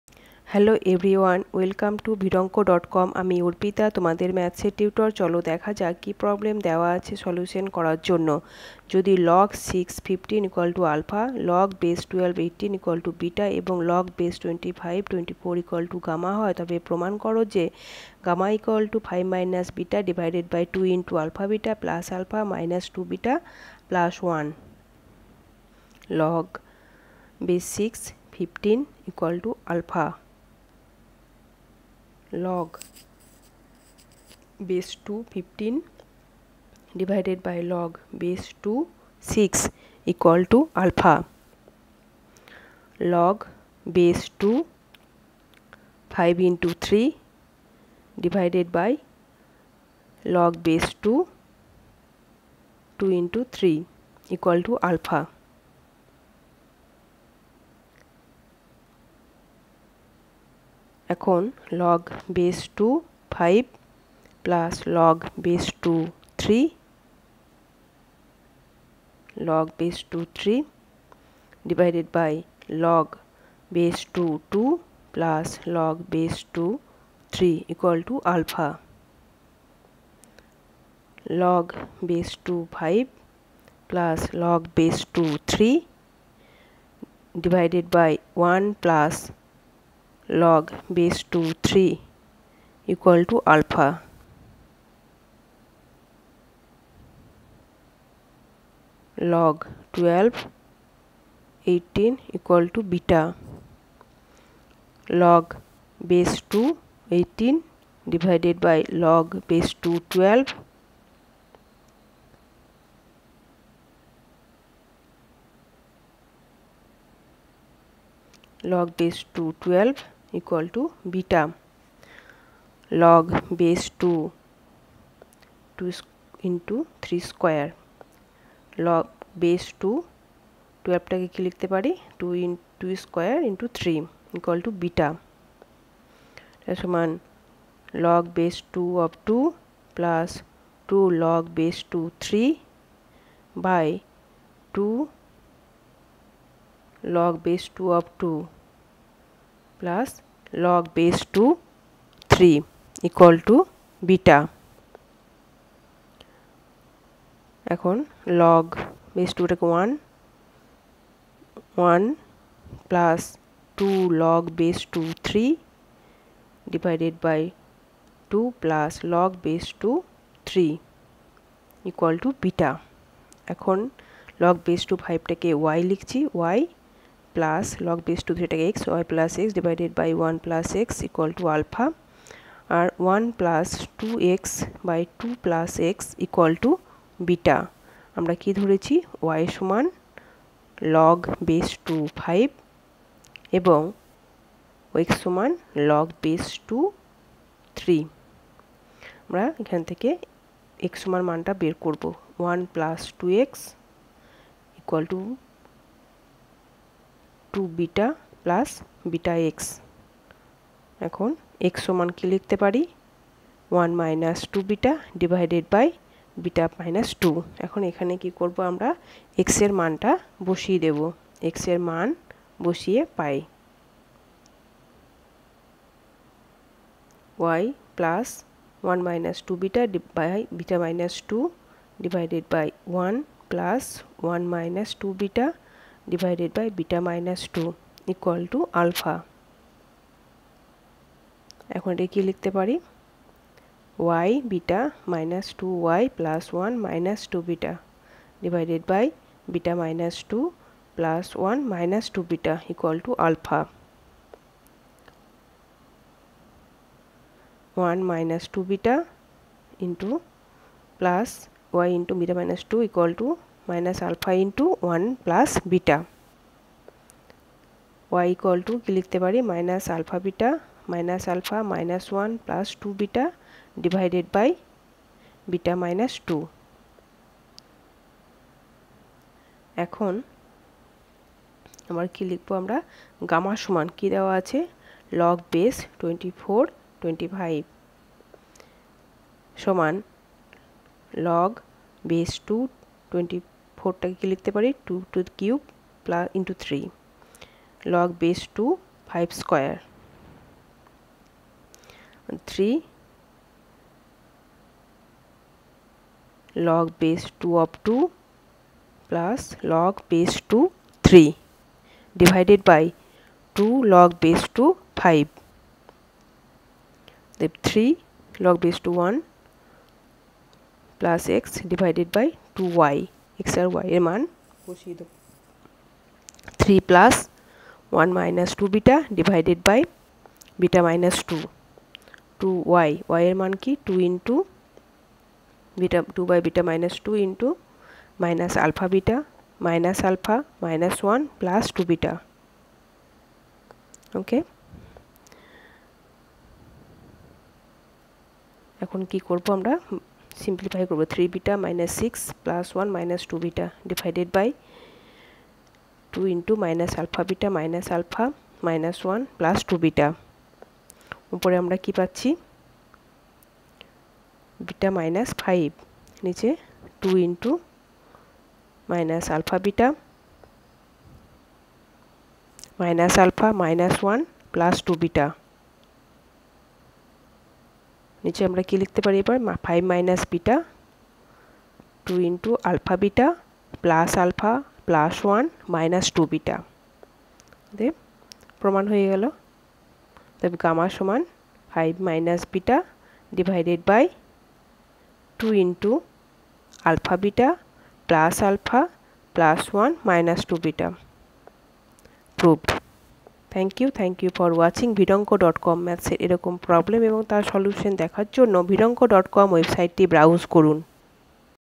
West Bengal Board Class 9 Maths Book Solution in Bengali - K C Nag Anushiloni Question: 7.5(b)
Get free Maths book video and PDF solutions in Bengali for K C Nag. Free answer and explanation through video for question 7.5(b), of Chapter Logarithm from K C Nag followed for Class 9, West Bengal Board of Secondary Education (WBBSE).

Topic of discussion in this video:

    K C Nag Book Solution
    West Bengal Board Class 9 Math Book Solution
    Logarithm